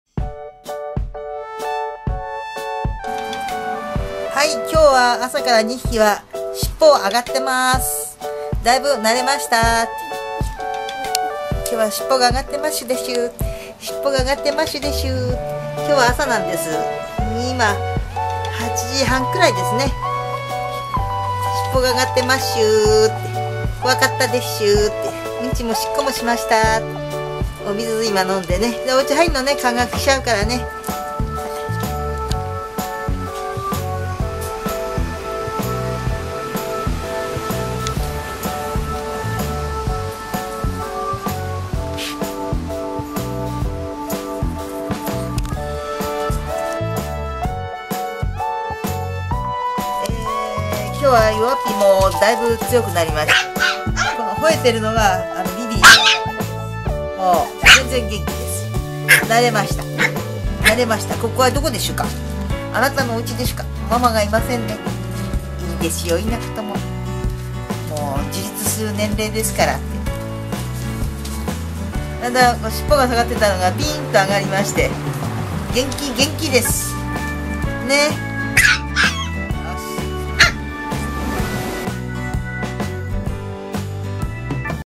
我が家に来て、２日目と３日目。だいぶ慣れたよ！
NO２．柴犬の子犬の兄弟、生後５０日。
我が家に来て３日目環境に慣れてきました。
初日は尻尾が下がった子犬もしっかり巻き上げてます。